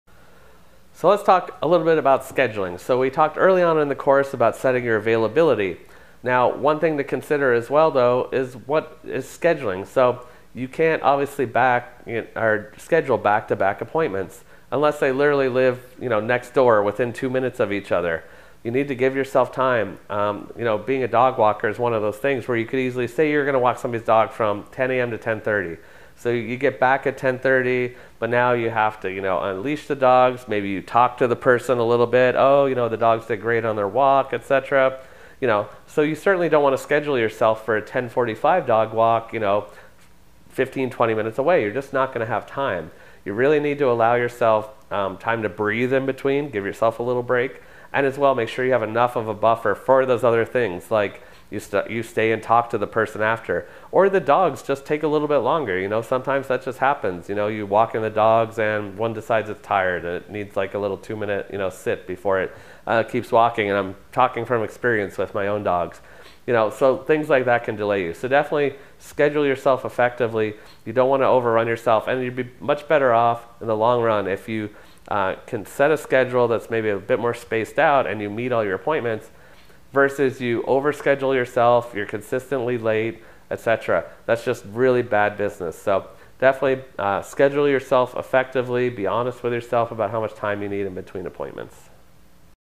Business 101: Start A Dog Walking Business : Scheduling effectively
Business 101: Start A Dog Walking Business
Everything you need to know to start your dog walking business today!